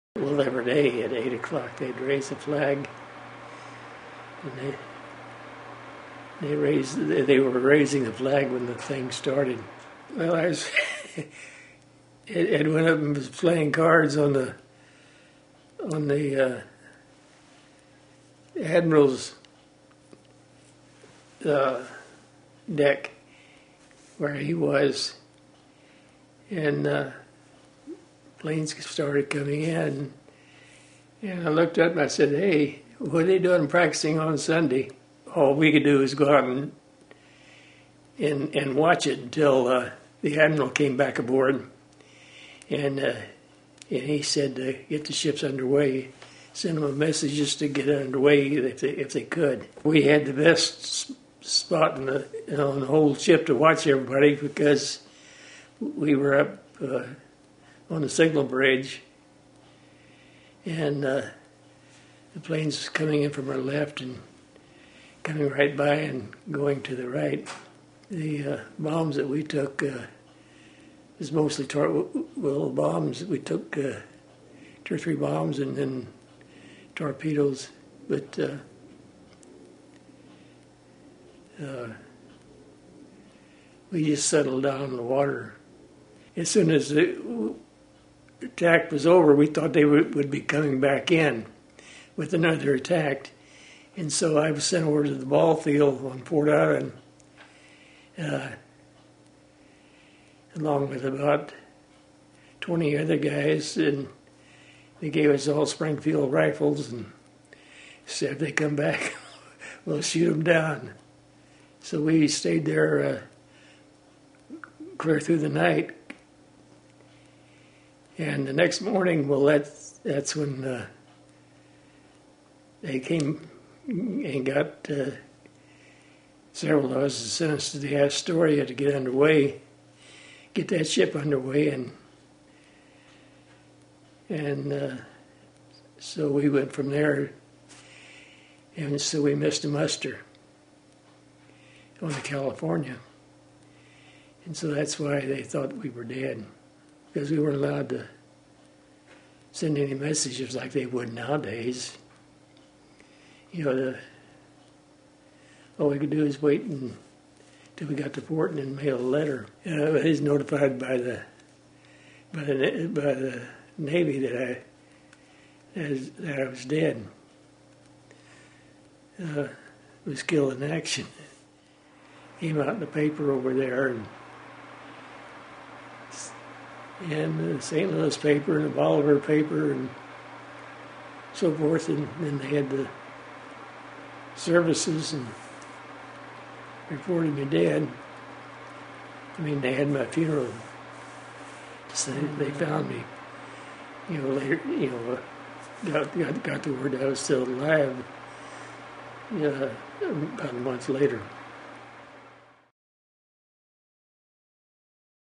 Pearl Harbor Survivor Fred Ray
After the attack, Ray was sent to help USS Astoria get underway, and missed muster on the California. The Navy thought he was dead, and notified his family. Local newspapers published his obituary and accounts of his memorial service. "We missed the muster on the California, so that's why they thought we were dead," he said. " We weren't allowed to send any messages like they would nowadays. All we could do is wait until we got to port and mail a letter."